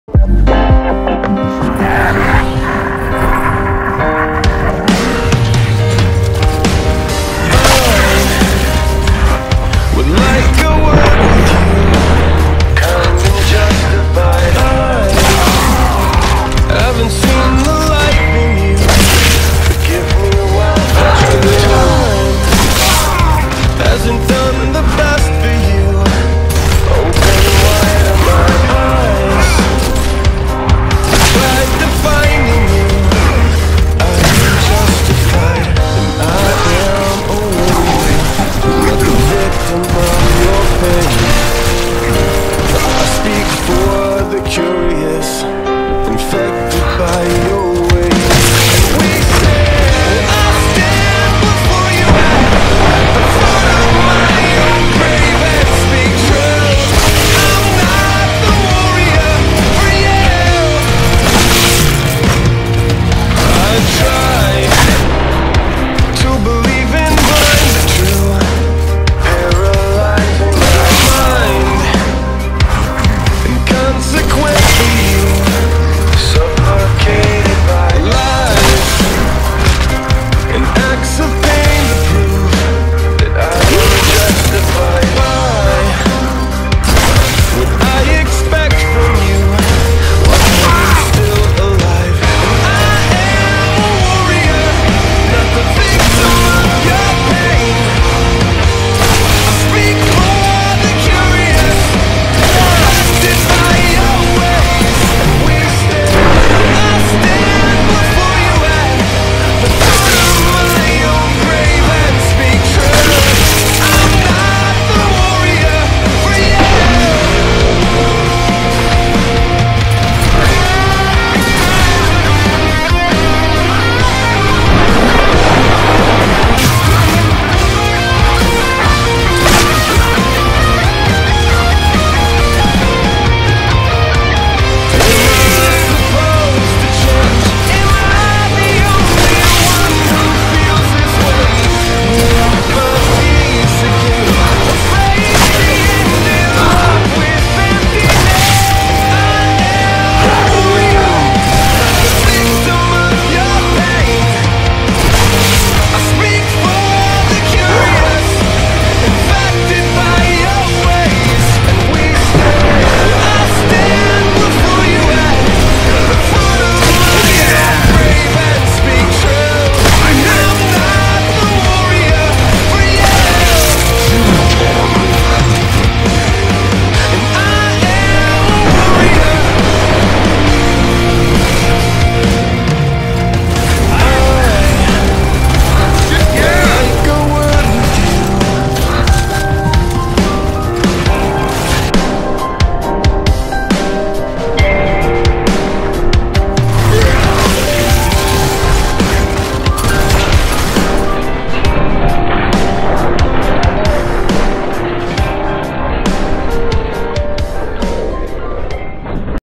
Taken MiLkBoNe Episode I AM A WARRIOR !!!!!
Hey guys thought I would make you a little montage of some of my previous better clips that I had sitting on my computer, hope you all enjoy and thanks for all your support.